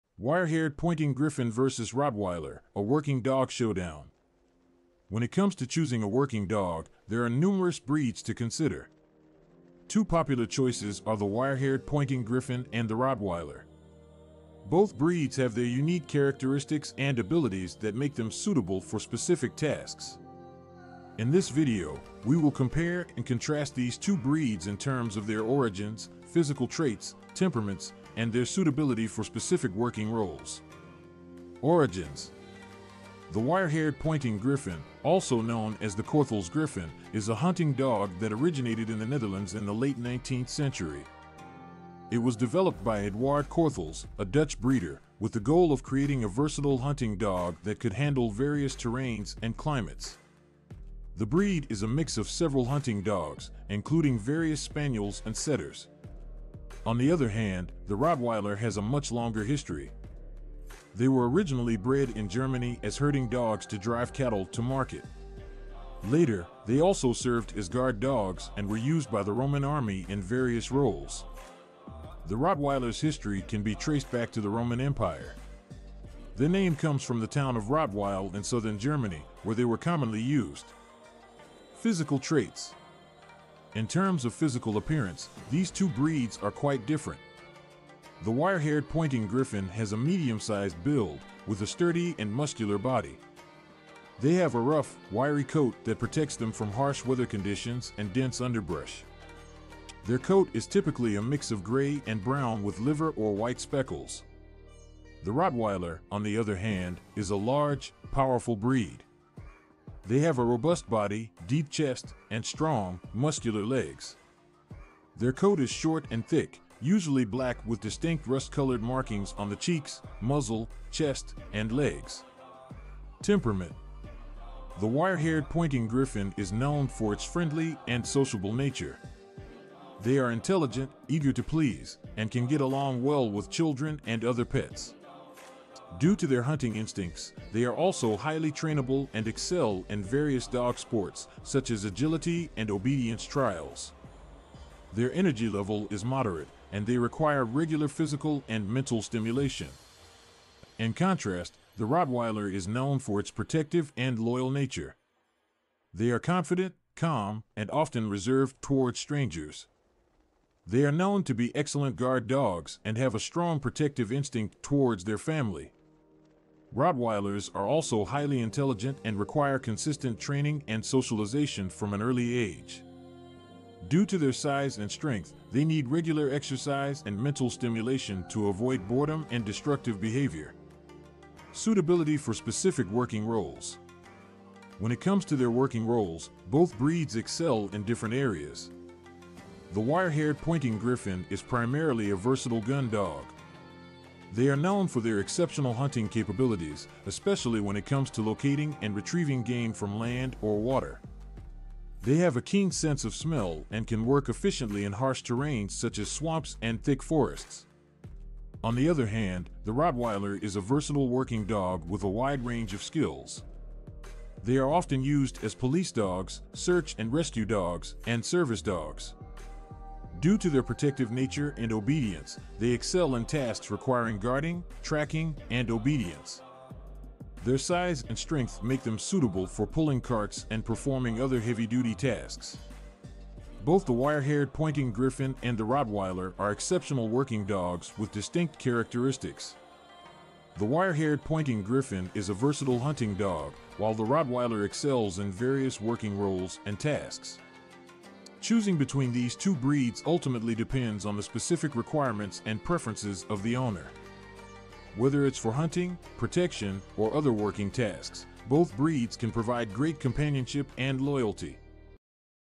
The Working Dog Showdown: Wirehaired Pointing Griffon vs Rottweiler
Wirehaired Pointing Griffon vs Rottweiler: A Working Dog Showdown
When it comes to choosing a working dog, there are numerous breeds to consider
 Two popular choices are the Wirehaired Pointing Griffon and the Rottweiler
 Both breeds have their unique characteristics and abilities that make them suitable for specific tasks
 In this video, we will compare and contrast these two breeds in terms of their origins, physical traits, temperaments, and their suitability for specific working roles
Origin